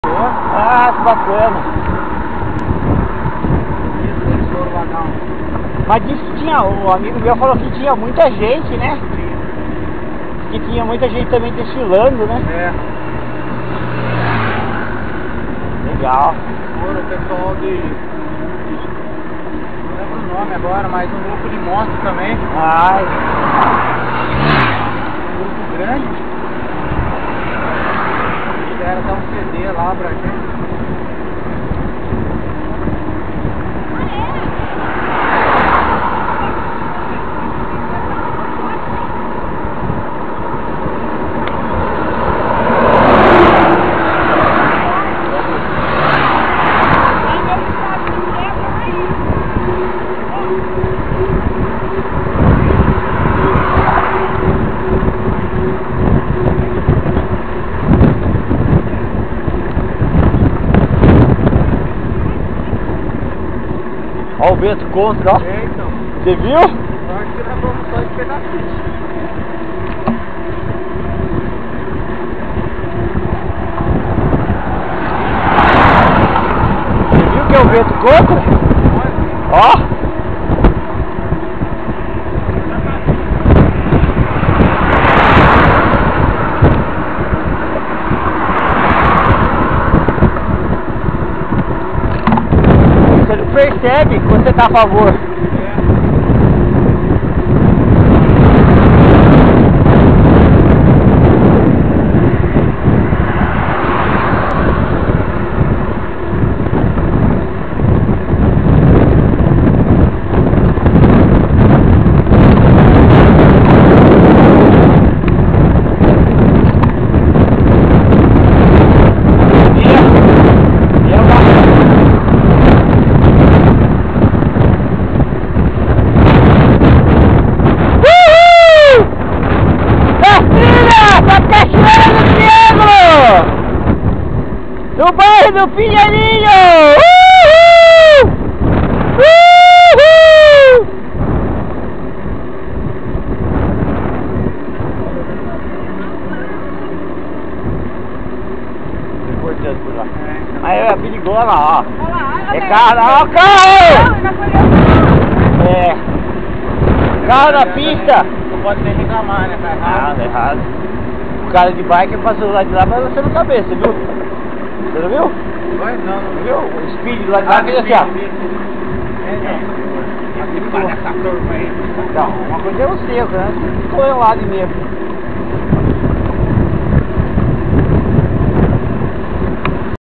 Full HD, Mtb, Trilha, Cachoeira do Triângulo, 26 amigos, 49 km, Taubate, 2016, 134
Venha pedalar conosco, vamos pedalar,  vamos conhecer as cachoeiras da Serra da Mantiqueira.
4k, Ultra HD, Full HD, Mountain bike, Taubaté, SP, Brasil, 10 de Setembro de 2016.